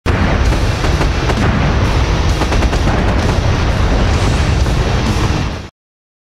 MAG - Dom - Call the Cops?
What do you think? What caused this?

I'm guessing the designated drivers didn't take the keys away...and the drivers had one too many drinks celebrating the last match...